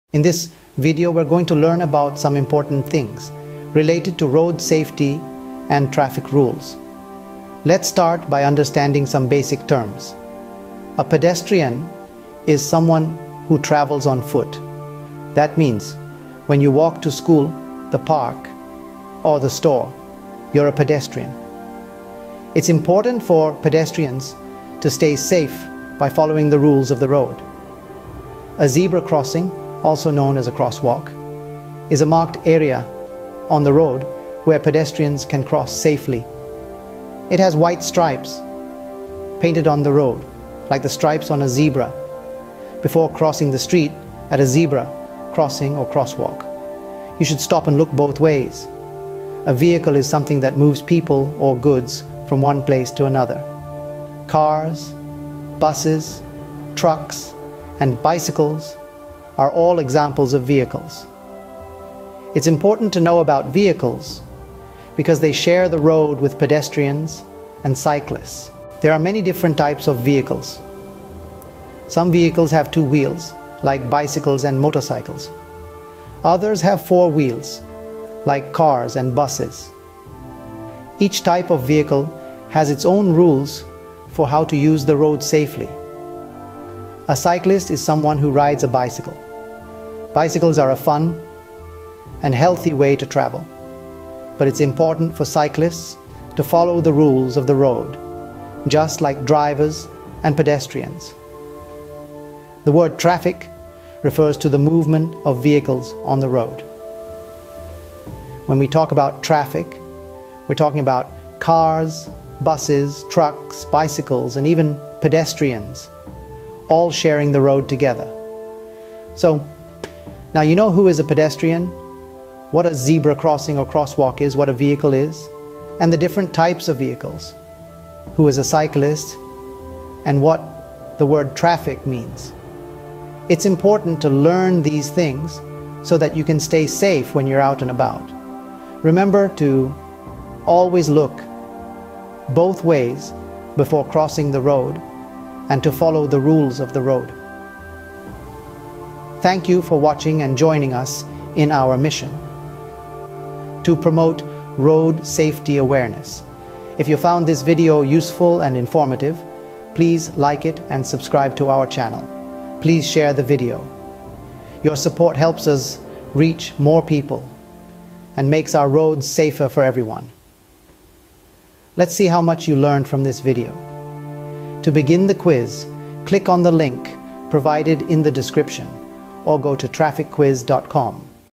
Ultimate Guide to Road Safety for kids Part 1
Hey, young explorers! Are you ready to become a Road Safety Superhero? 🦸‍♂️🦸‍♀️ Join us on an exciting adventure where we learn all about staying safe on the road. 🌟

👣 Who’s a Pedestrian? - Discover how to be a smart walker in the bustling world of sidewalks and crossings! 🛑 Pedestrian Safety Tips - Master the art of crossing streets with our super-secret safety tips that even adults don’t know! 🚘 What are Vehicles? - Zoom into the world of cars, buses, and trucks and learn what makes them go vroom! 🚲 Who’s a Cyclist? - Put on your helmets as we pedal through the dos and don’ts of bicycle safety! 🚦 What is Traffic? - Decode the mystery of traffic lights and signs to become the wisest kid on the block!

Whether you’re walking, cycling, or just out for a ride, we’ve got the coolest tips and tricks to keep you safe and sound. So, buckle up and get ready for a journey that’s as fun as it is educational—because safety never takes a backseat! 🎒🛴